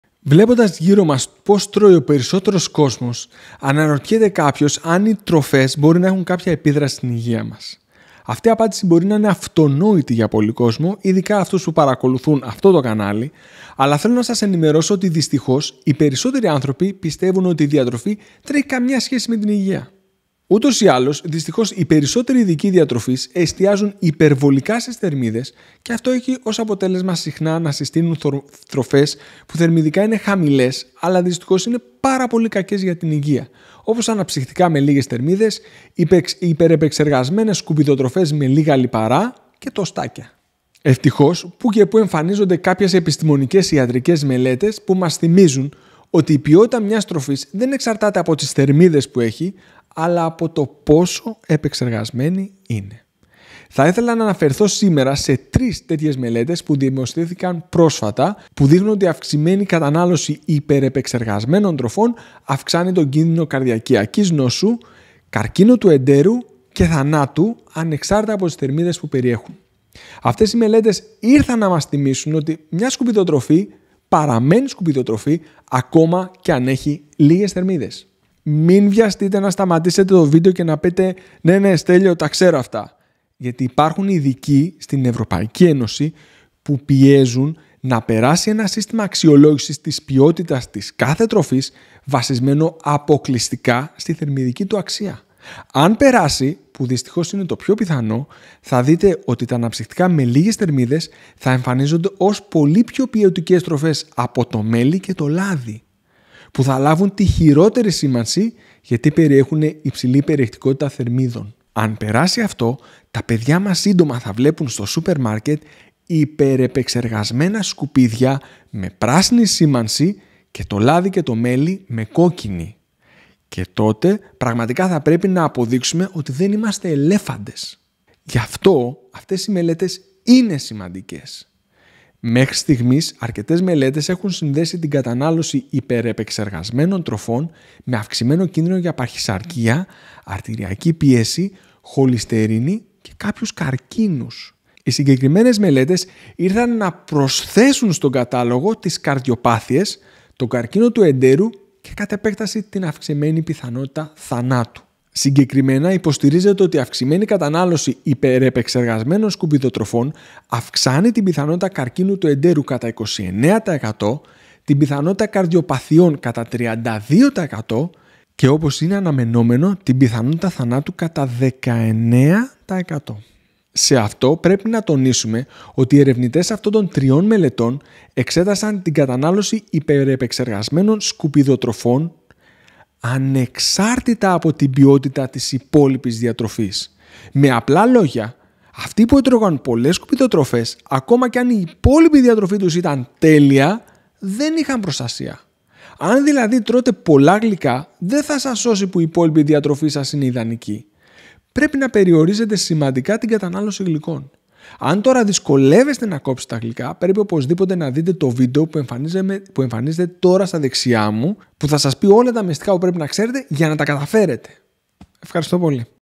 Τι θα συμβεί αν τρώτε συχνά γλυκά
Βλέποντας γύρω μας πως τρώει ο περισσότερος κόσμος, αναρωτιέται κάποιος αν οι τροφές μπορεί να έχουν κάποια επίδραση στην υγεία μας. Αυτή η απάντηση μπορεί να είναι αυτονόητη για πολύ κόσμο, ειδικά αυτούς που παρακολουθούν αυτό το κανάλι, αλλά θέλω να σας ενημερώσω ότι δυστυχώς οι περισσότεροι άνθρωποι πιστεύουν ότι η διατροφή δεν έχει καμία σχέση με την υγεία.

Ούτως ή άλλως, δυστυχώς οι περισσότεροι ειδικοί διατροφής εστιάζουν υπερβολικά στις θερμίδες και αυτό έχει ως αποτέλεσμα συχνά να συστήνουν τροφές που θερμιδικά είναι χαμηλές, αλλά δυστυχώς είναι πάρα πολύ κακές για την υγεία, όπως αναψυκτικά με λίγες θερμίδες, υπερ-επεξεργασμένες σκουπιδοτροφές με λίγα λιπαρά και τοστάκια. Ευτυχώς, που και που εμφανίζονται κάποιες επιστημονικές ιατρικές μελέτες που μας θυμίζουν ότι η ποιότητα μιας τροφής δεν εξαρτάται από τις θερμίδες που έχει, αλλά από το ποσό επεξεργασμένη είναι.

Θα ήθελα να αναφερθώ σήμερα σε τρεις τέτοιες μελέτες που δημοσιεύτηκαν πρόσφατα που δείχνουν ότι η αυξημένη κατανάλωση υπερ-επεξεργασμένων τροφών αυξάνει τον κίνδυνο καρδιαγγειακής νόσου, καρκίνου του εντέρου και θανάτου, ανεξάρτητα από τις θερμίδες που περιέχουν. Αυτές οι μελέτες ήρθαν να μας θυμίσουν ότι μία σκουπιδοτροφή παραμένει σκουπιδοτροφη ακόμα και αν έχει λίγες θερμίδες.

Μην βιαστείτε να σταματήσετε το βίντεο και να πείτε, “ Ναι, ναι Στέλιο, τα ξέρω αυτά”, γιατί υπάρχουν ειδικοί στην Ευρωπαϊκή Ένωση που πιέζουν να περάσει ένα σύστημα αξιολόγησης της ποιότητας της κάθε τροφής βασισμένο αποκλειστικά στη θερμιδική του αξία. Αν περάσει, που δυστυχώς είναι το πιο πιθανό, θα δείτε ότι τα αναψυκτικά με λίγες θερμίδες θα εμφανίζονται ως πολύ πιο ποιοτικές τροφές από το μέλι και το λάδι, που θα λάβουν τη χειρότερη σήμανση γιατί περιέχουν υψηλή πυκνότητα θερμίδων. Αν περάσει αυτό, τα παιδιά μας σύντομα θα βλέπουν στα σούπερ μάρκετ υπέρ-επεξεργασμένα σκουπίδια με πράσινη σήμανση και το λάδι και το μέλι με κόκκινη. Και τότε πραγματικά θα πρέπει να αποδείξουμε ότι δεν είμαστε ελέφαντες.

Για αυτό αυτές οι μελέτες είναι σημαντικές. Μέχρι στιγμής αρκετές μελέτες έχουν συνδέσει την κατανάλωση υπερ-επεξεργασμένων τροφών με αυξημένο κίνδυνο για παχυσαρκία, αρτηριακή υπέρταση, χοληστερίνη και κάποιους καρκίνους. Οι συγκεκριμένες μελέτες ήρθαν να προσθέσουν στον κατάλογο τις καρδιοπάθειες, τον καρκίνο του εντέρου και κατ΄επέκταση την αυξημένη πιθανότητα θανάτου. Συγκεκριμένα, υποστηρίζεται ότι η αυξημένη κατανάλωση υπερ-επεξεργασμένων σκουπιδοτροφών αυξάνει την πιθανότητα καρκίνου του εντέρου κατά 29%, την πιθανότητα καρδιοπαθειών κατά 32%, και όπως είναι αναμενόμενο, την πιθανότητα θανάτου κατά 19%.

Σε αυτό πρέπει να τονίσουμε ότι οι ερευνητές αυτών των τριών μελετών εξέτασαν την κατανάλωση υπέρ-επεξεργασμένων σκουπιδοτροφών, ανεξάρτητα από την ποιότητα της υπόλοιπης διατροφής. Με απλά λόγια, αυτοί που έτρωγαν πολλες σκουπιδοτροφές, ακόμα και αν η υπόλοιπη διατροφή τους ήταν τέλεια, δεν είχαν προστασία. Αν δηλαδή τρώτε πολλά γλυκά, δεν θα σας σώσει που η υπόλοιπη διατροφή σας είναι ιδανική. Πρέπει να περιορίσετε σημαντικά την κατανάλωση γλυκών. Αν τώρα δυσκολεύεστε να κόψετε τα γλυκά, πρέπει οπωσδήποτε να δείτε το βίντεο που εμφανίζεται τώρα στα δεξιά μου που θα σας πει όλα τα μυστικά που πρέπει να ξέρετε για να τα καταφέρετε. Ευχαριστώ πολύ.